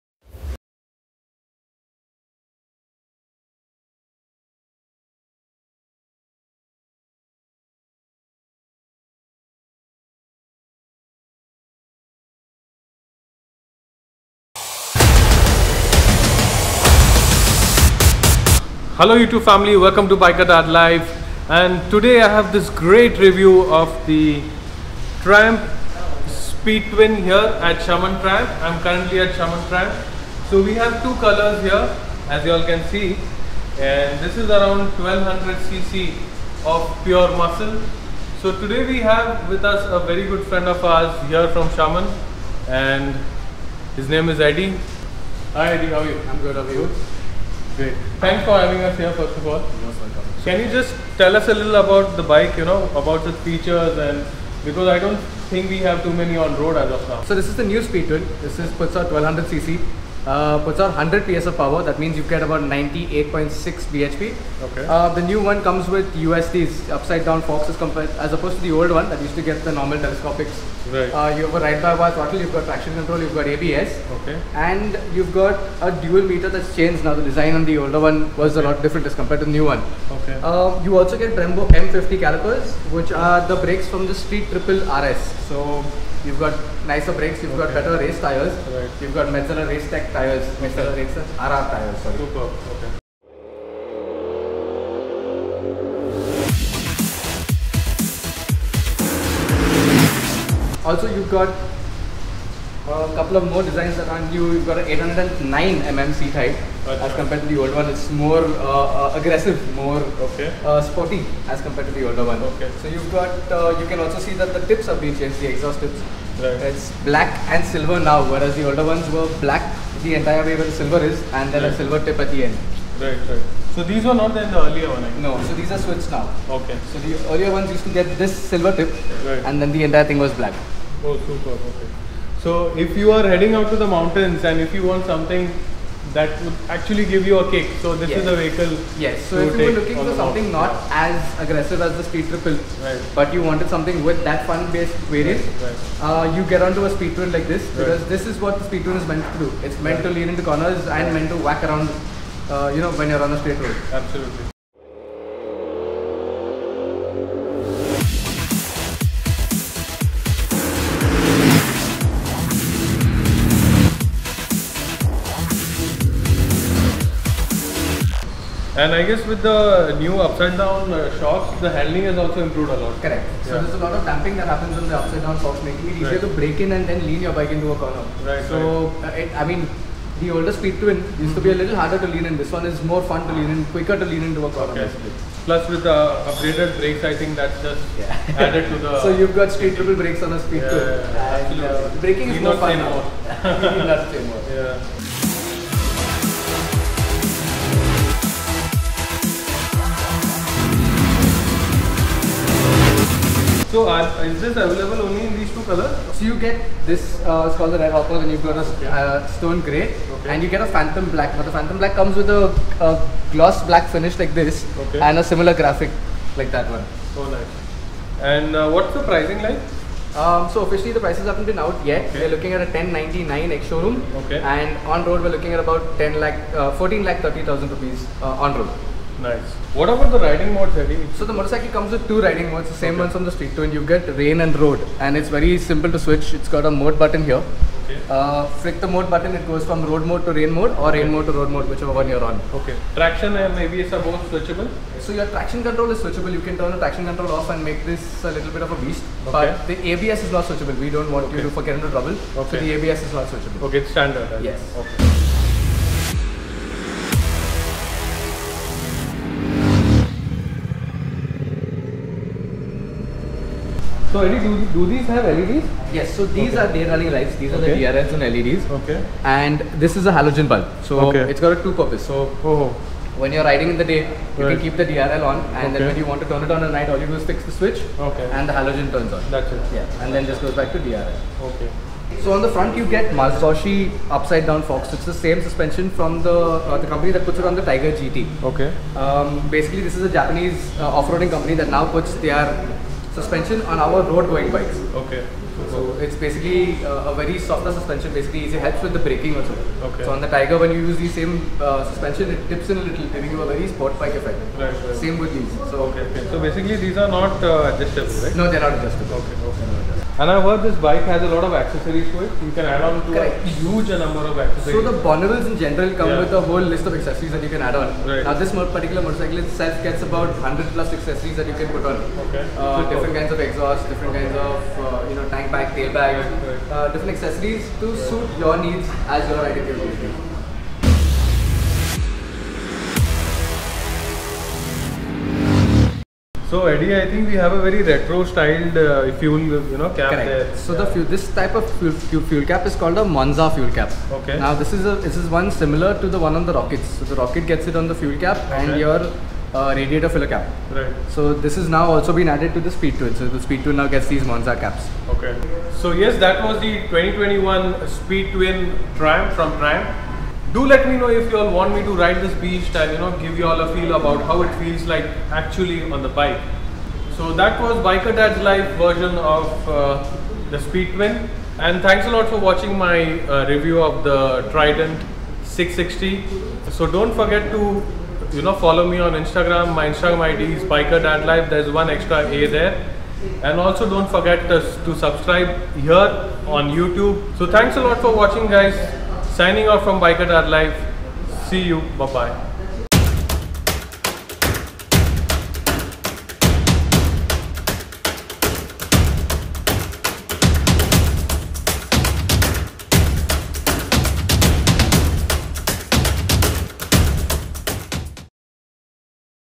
New 2021 Triumph Speed Twin BS 6 Review┃Detailed Features┃BikerDaadLife
Hello Family,

Have tried to covered most of the features of the Triumph Speed Twin in this video. Hope you all like it !!

Would like to thank Shaman Triumph and their team at Andheri Showroom for lending the bike to me !!

Note : Video footage taken from Official Triumph Motorcycles. Thank you : )